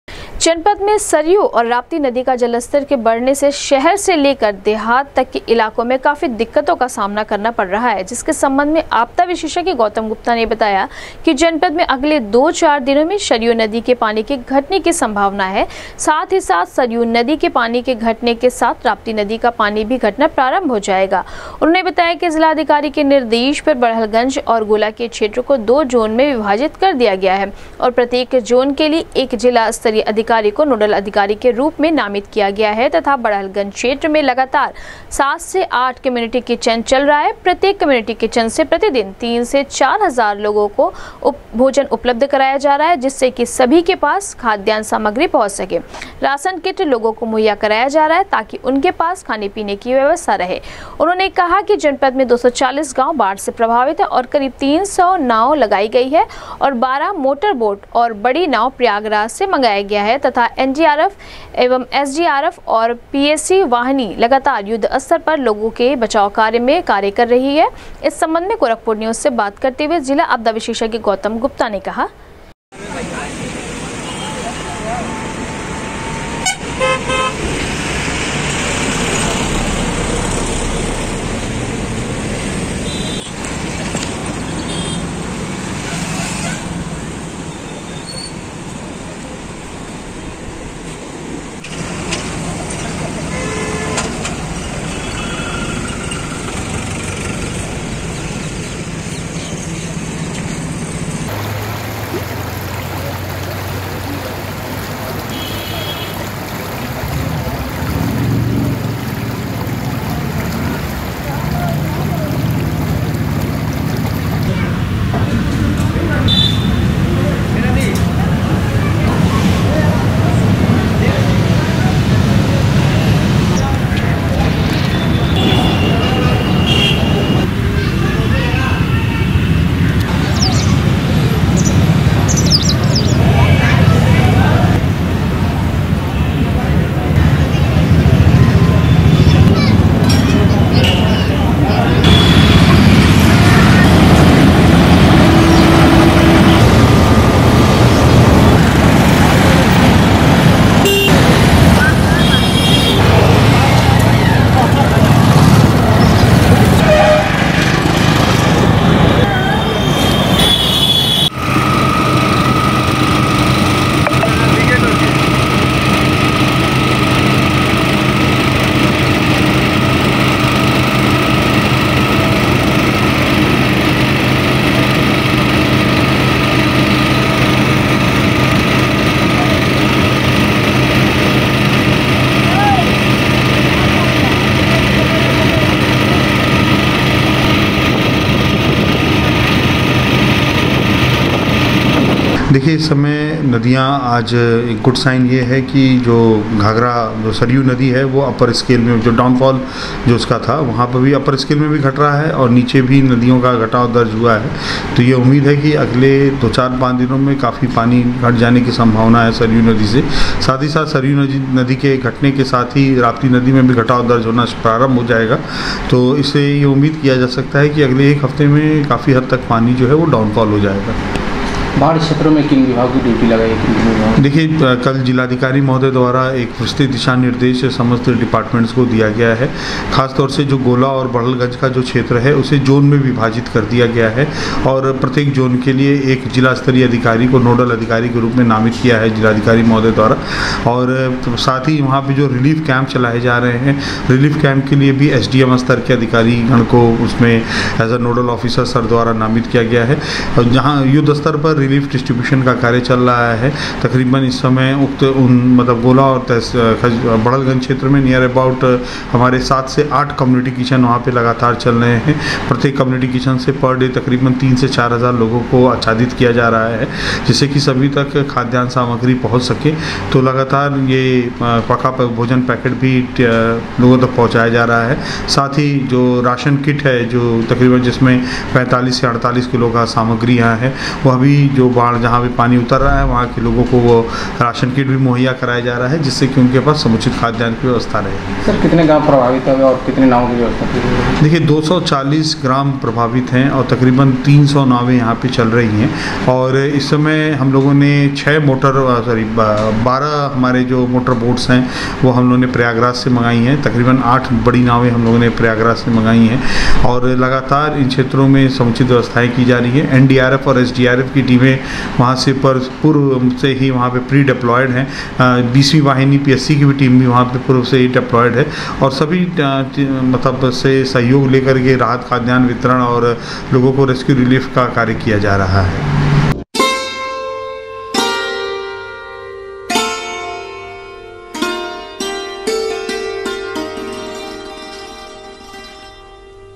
जनपद में अगले दो-चार दिनों में सरयू और राप्ती नदी के जलस्तर के घटने की है संभावना, बड़हलगंज और गोला
जनपद में अगले दो-चार दिनों में सरयू और राप्ती नदी के जलस्तर के घटने की है संभावना, बड़हलगंज और गोला को दो जोन में विभाजित कर जिला स्तरीय अधिकारी को नोडल अधिकारी के रूप में किया गया नामित, बाढ़ से प्रभावित 240 गांव में करीब 300 नाव और 12 मोटर बोट की सहायता से राहत व बचाव कार्य युद्वस्तर पर है जारी,जिला आपदा विशेषज्ञ ने दी जानकारी